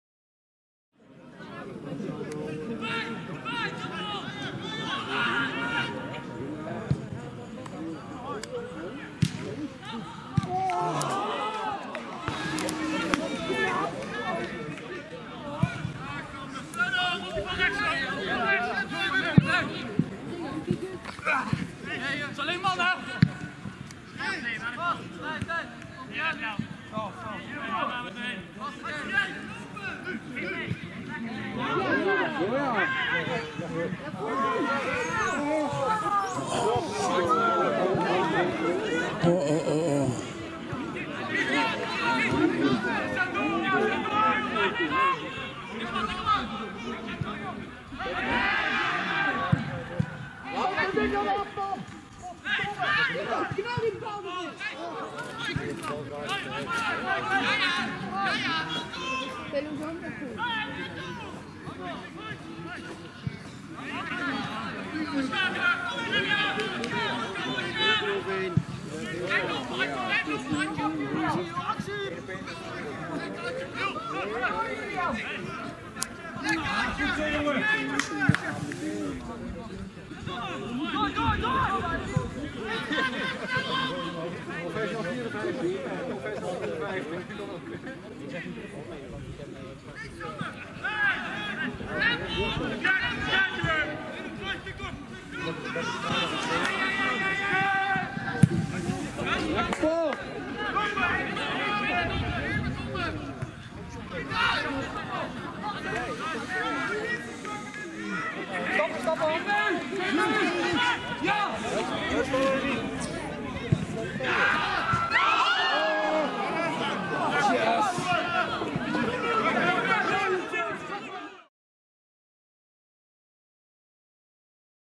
Veluwezoom 1 - Driel 1 (30 augustus 2020)
Pas in de 2e helft creëerde Driel goede kansen voor zichzelf, maar ze werden niet benut. Dat lukte Veluwezoom in de eindfase ook niet, al waren ze er dichtbij. Uiteindelijk bleef het duel zonder doelpunten. In deze video een paar spannende momenten in de 2e helft.